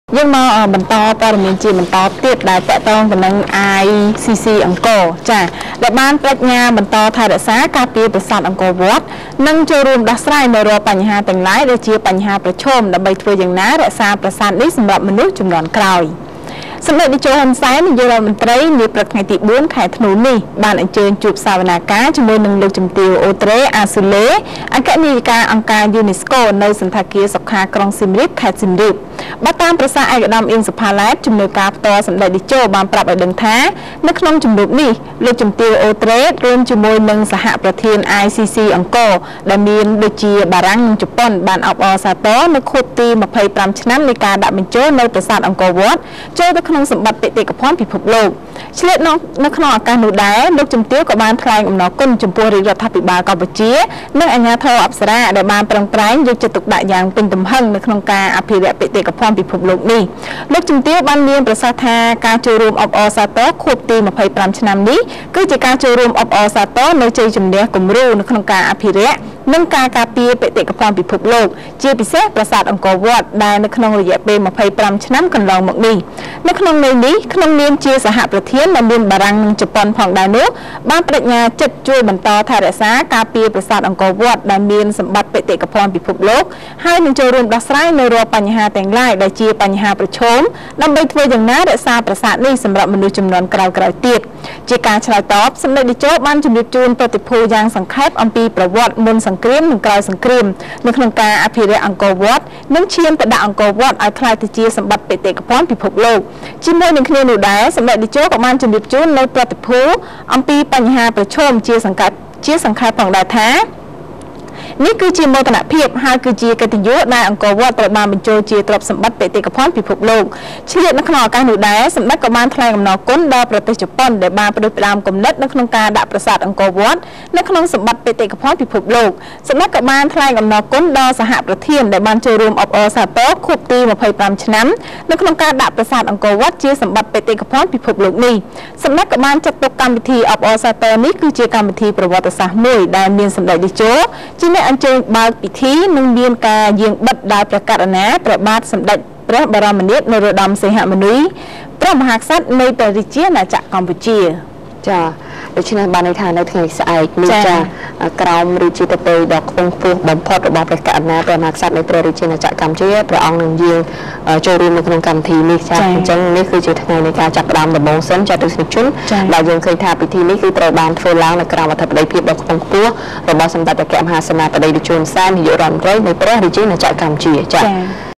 អាយស៊ីស៊ី-អង្គរ ប្តេជ្ញាបន្តថែរក្សា ការពារប្រាសាទអង្គរវត្ត និងចូលរួមដោះស្រាយ រាល់បញ្ហាទាំងឡាយ
អាយស៊ីស៊ី-អង្គរ ប្តេជ្ញាបន្តថែរក្សា ការពារប្រាសាទអង្គរវត្ត និងចូលរួមដោះស្រាយ រាល់បញ្ហាទាំងឡាយ ដែលជាបញ្ហាប្រឈម ដើម្បីធ្វើយ៉ាងណា រក្សាប្រាសាទនេះ សម្រាប់មនុស្សជំនាន់ក្រោយ